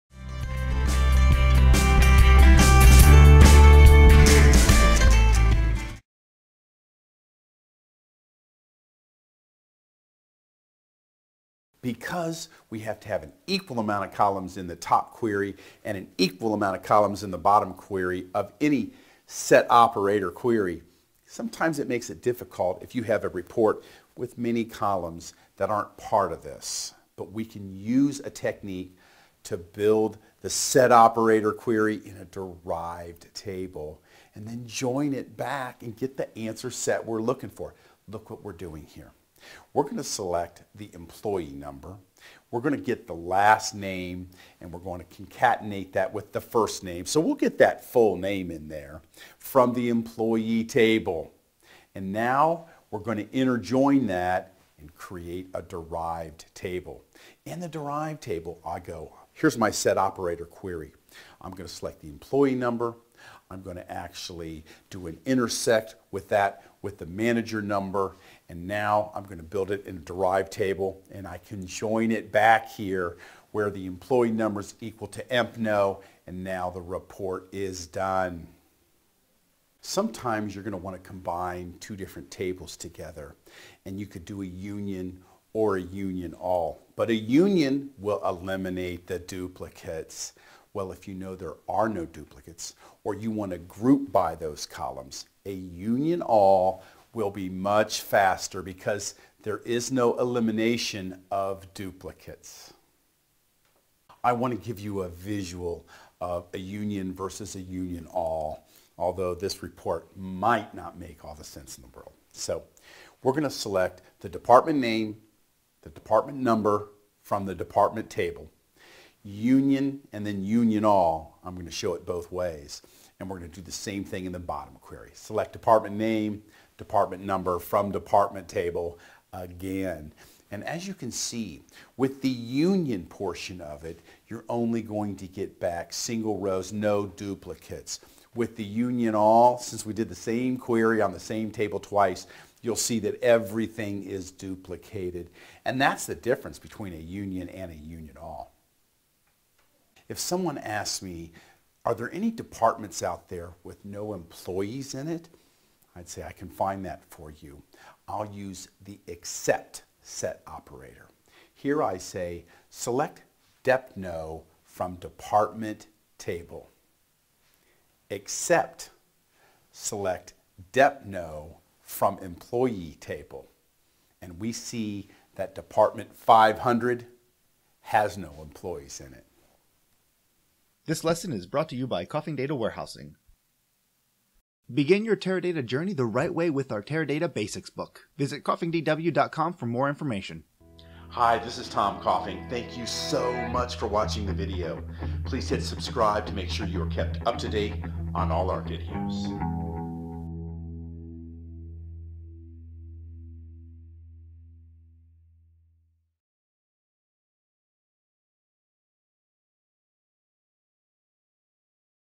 Let's Learn SQL! Lesson 117: Tricks and Examples of Set Operators
Explore SQL with Tom Coffing of Coffing Data Warehousing! In this lesson, check out these amazing tricks and examples of Set Operators!